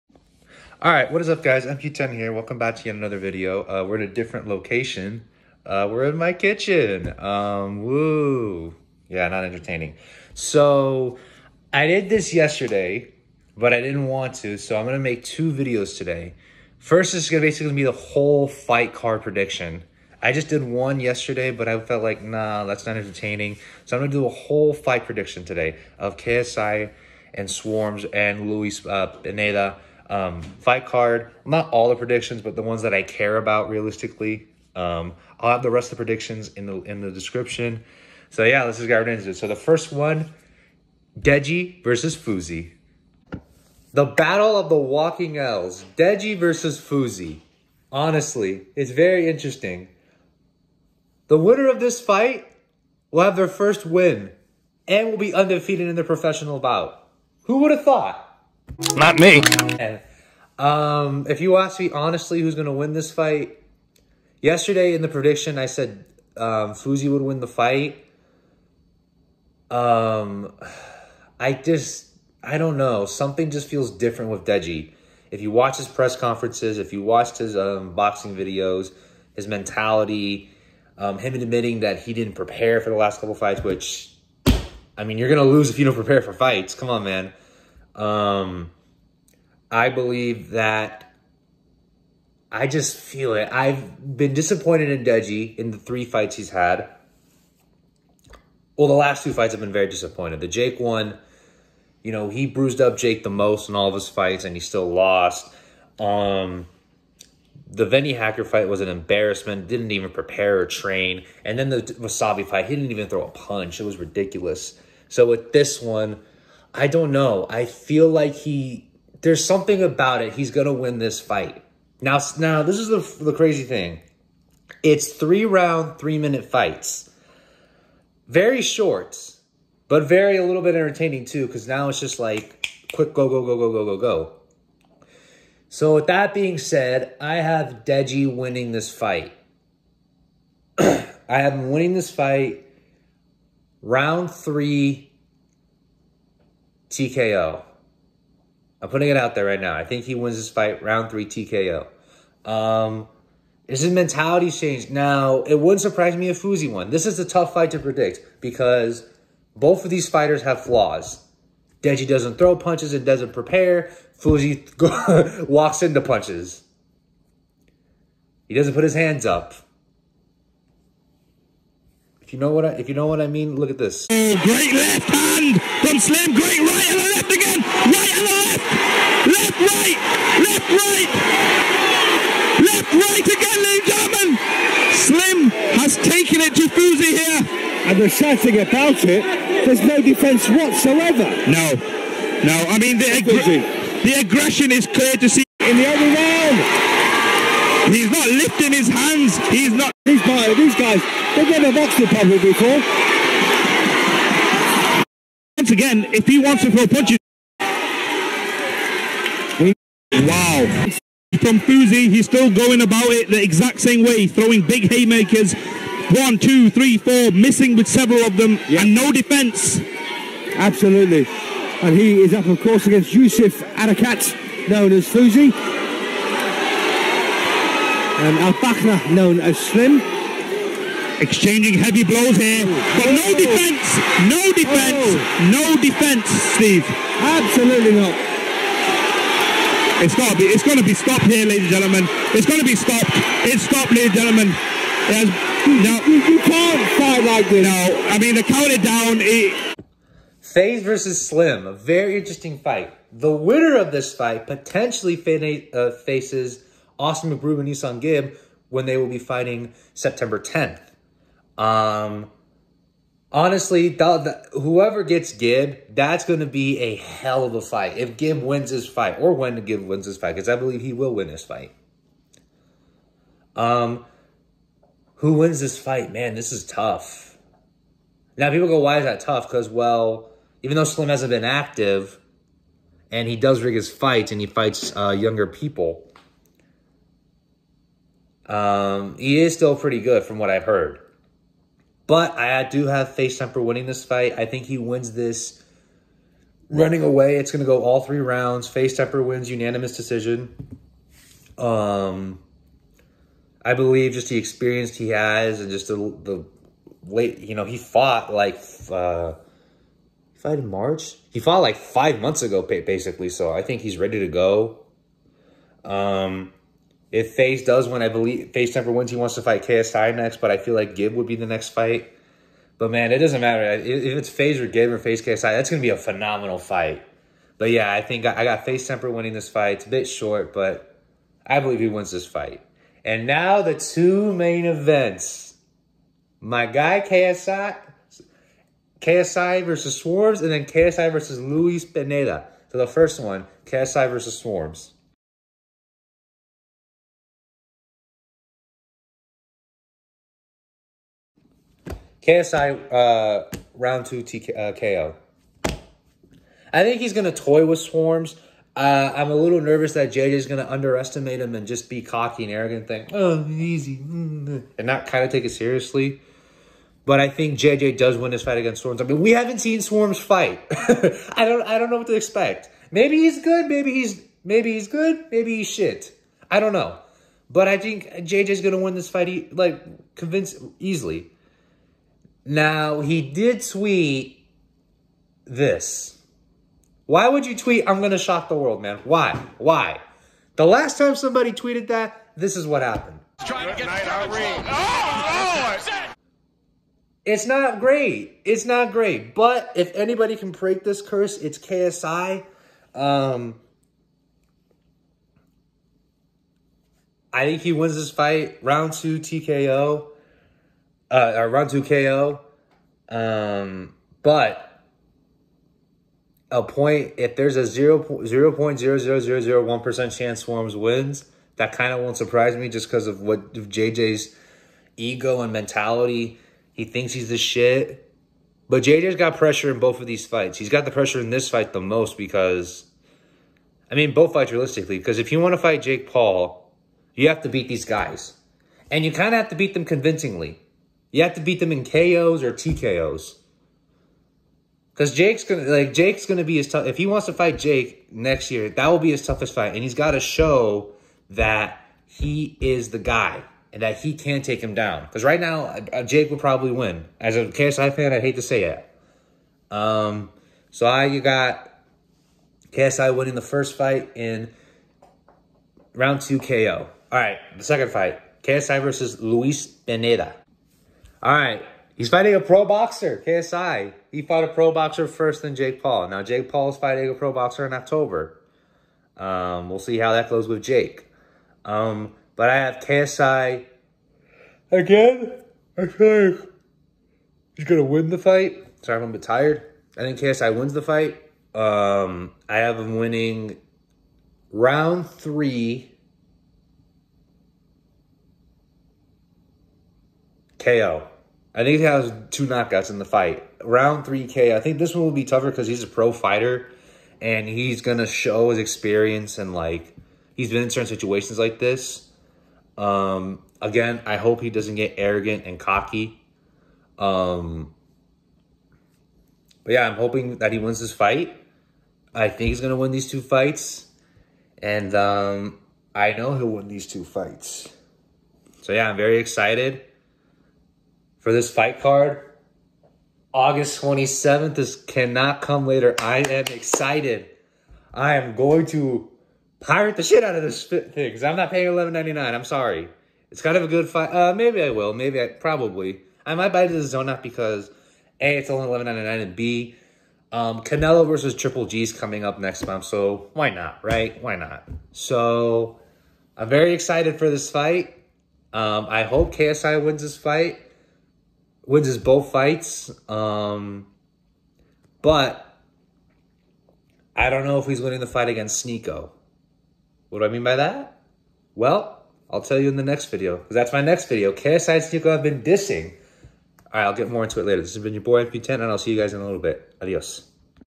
KSI VS SMARMZ AND LUIS PINEDA PREDICTIONS, the rest in the description
All my fight predictions
Sam Hyde KO win round 3 vs Iamthmpsn
Salt Papi KO win round 2 vs Andy Warski
Dee the great Split decision win vs Evil hero
King Kenny TKO win round 3 vs Faze Sensei
Deji TKO win round 3 over Fousey
Faze temper Split decision win over slim
KSI KO win Round 2 over SWARMZ
KSI KO win Round 2 over Luis Pineda